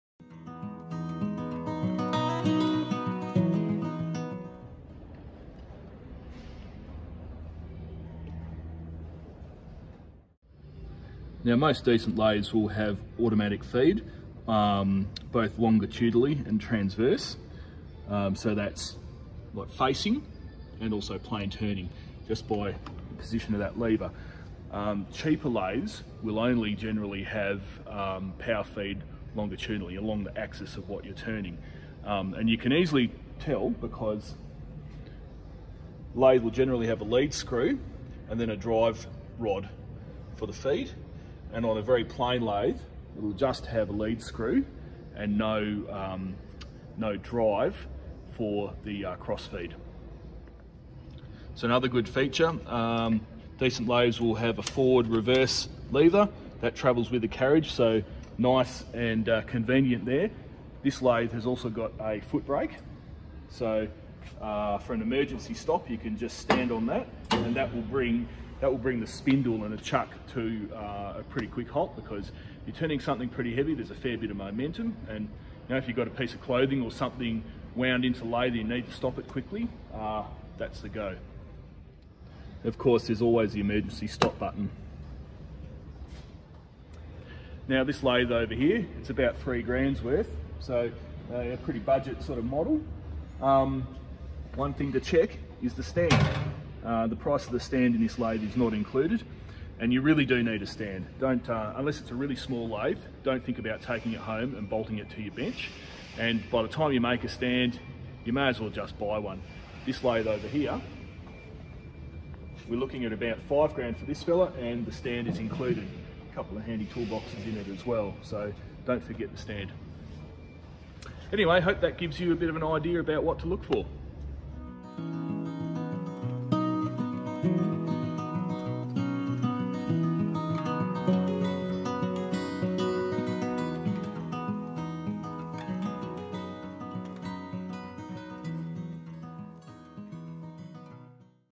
Buying a metal lathe Part 3 of 3
Kondinin Group engineer, Josh Giumelli explains what to look for when buying a metal lathe in this part three of a three-part series.